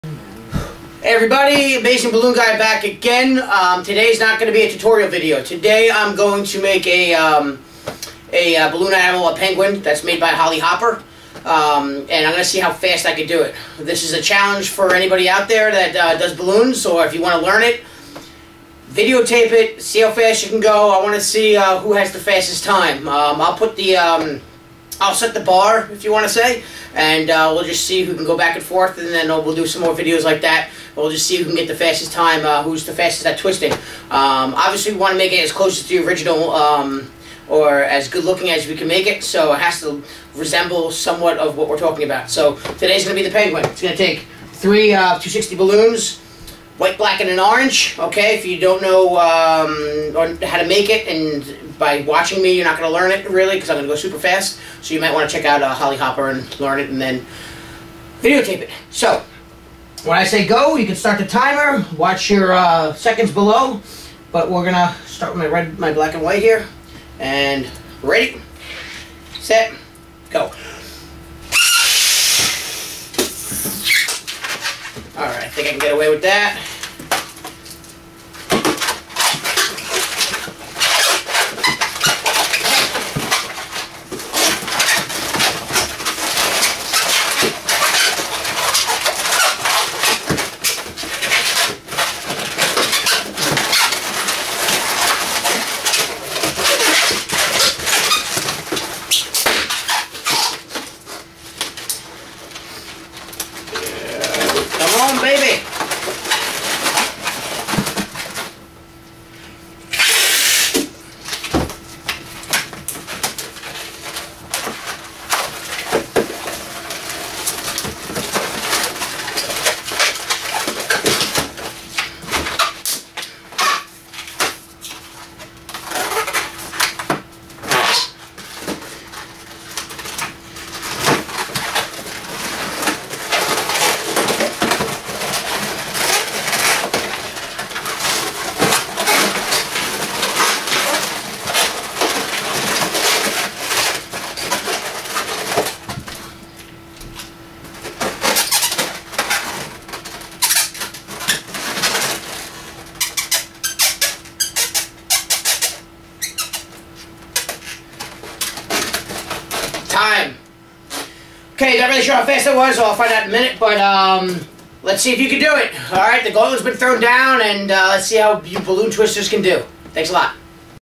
Penguin balloon speed challenge. Can you beat me?
Who can Twist a penguin balloon the fastest. This is  a Speed challenge for all you balloon twisters out there. Can you beat the Amazing Balloon Guy at twisting a balloon penguin?

watch this balloon decoration tutorial : "Cinderella Balloon Carriage Decoration  How to make a DIY Balloon decoration for Cinderella themed birthday"